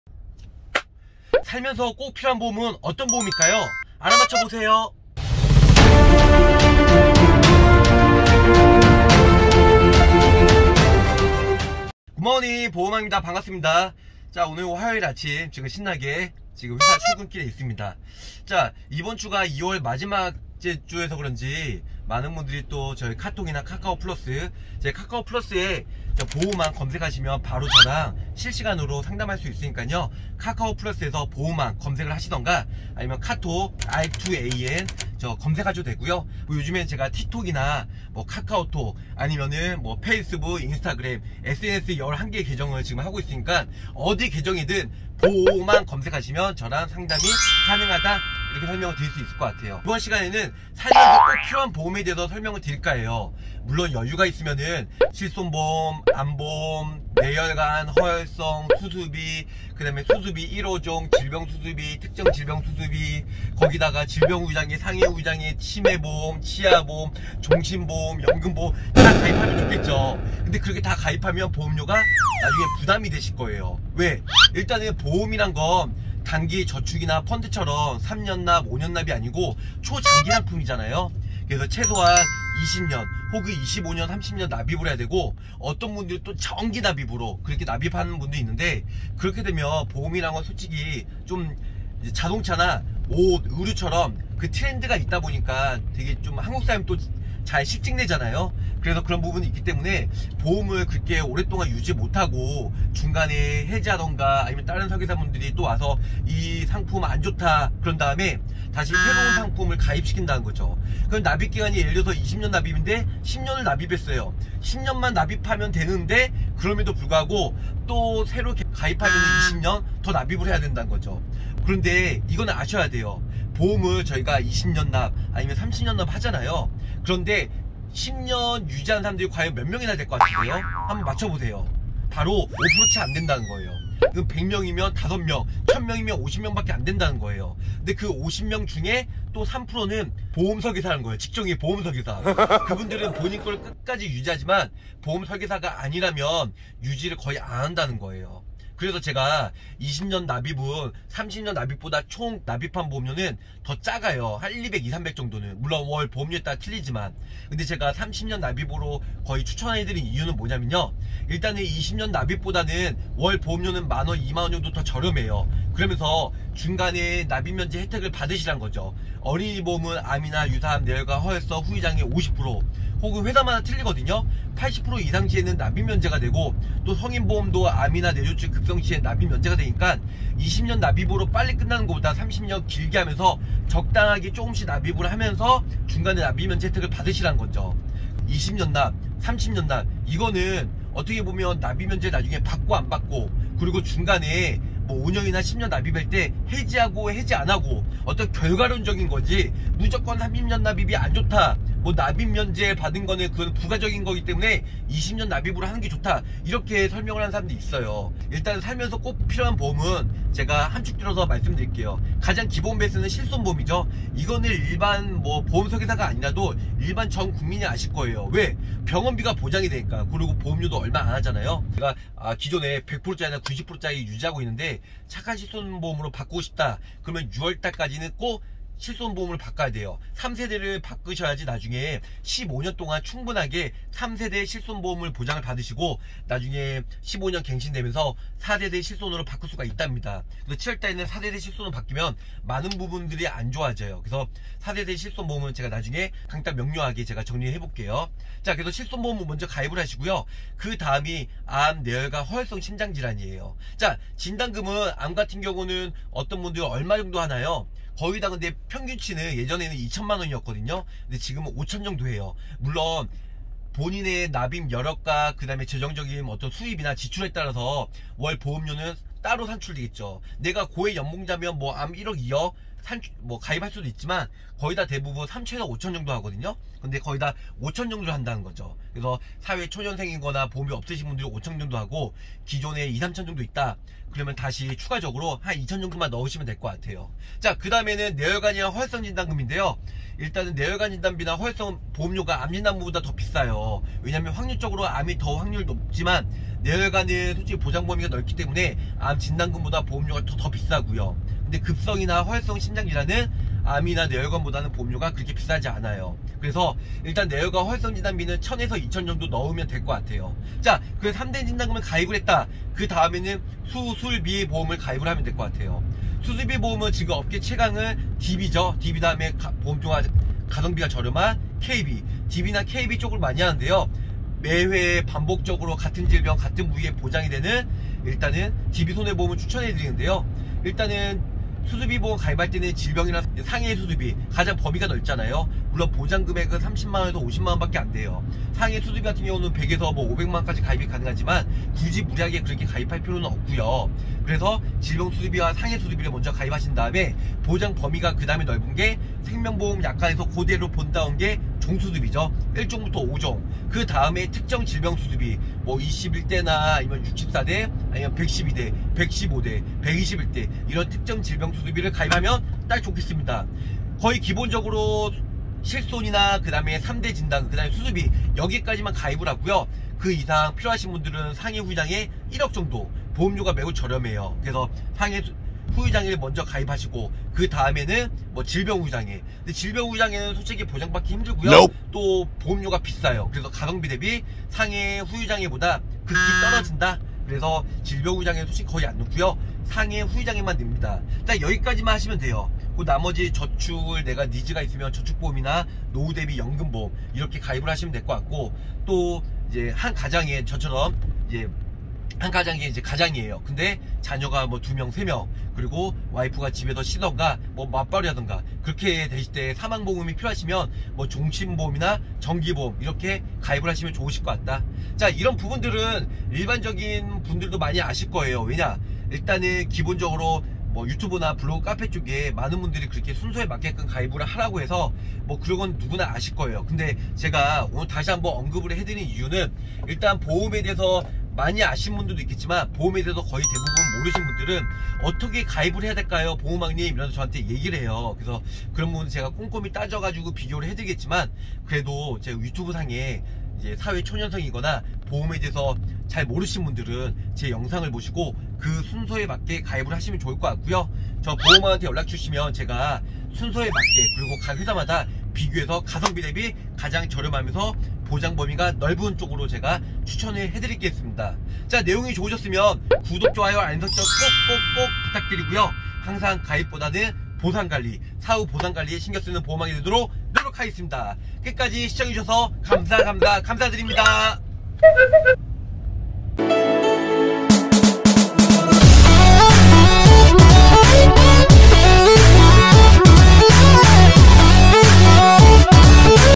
[보험왕] 살면서 꼭 필요한 보험은?
살면서 꼭 필요한 보험의 대해서 설명드립니다.
많은 도움되세요^^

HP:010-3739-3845
카톡친구추가:r2an
카톡플러스친구:보험왕